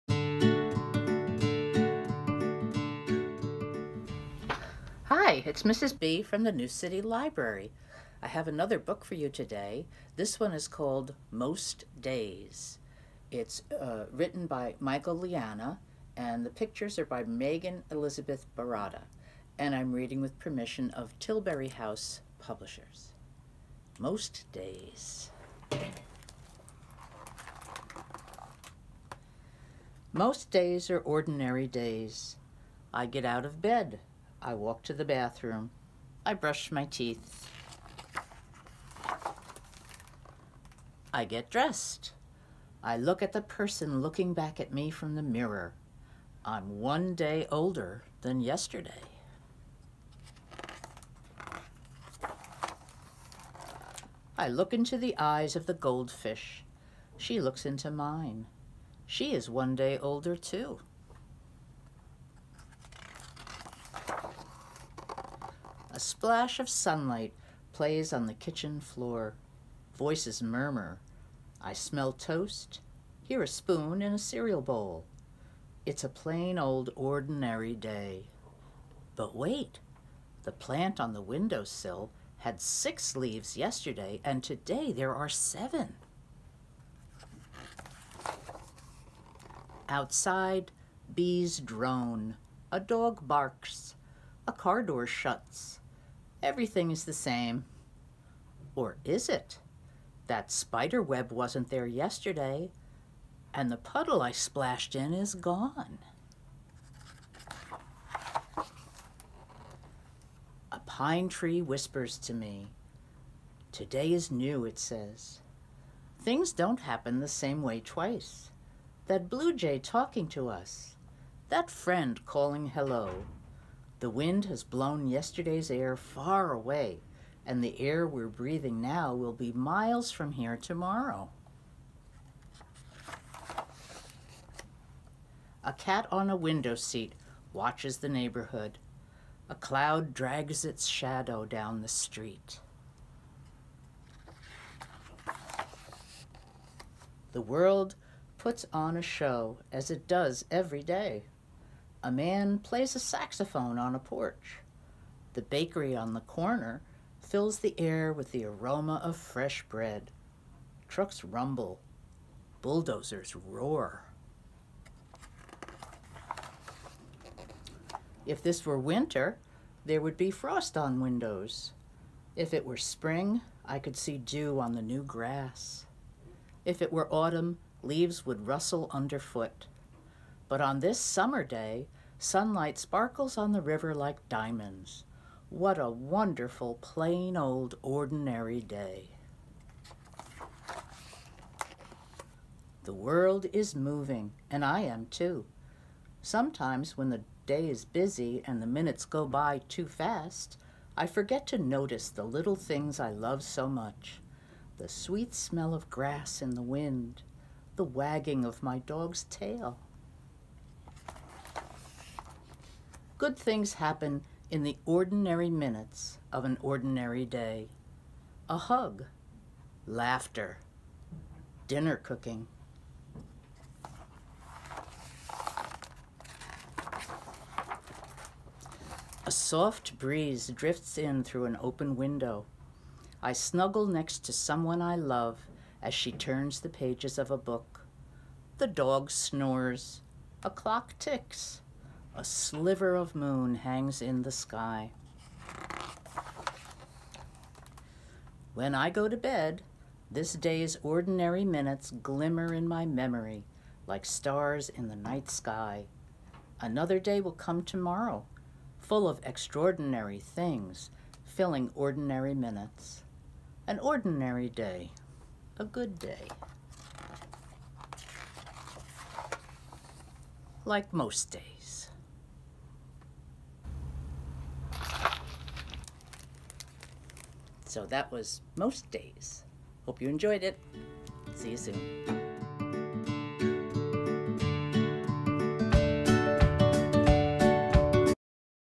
Virtual Storytime: Most Days
Mrs. B reads Most Days  by Michael Leannah  illustrated by Megan Elizabeth Baratta with permission of Tillbury House Publishers.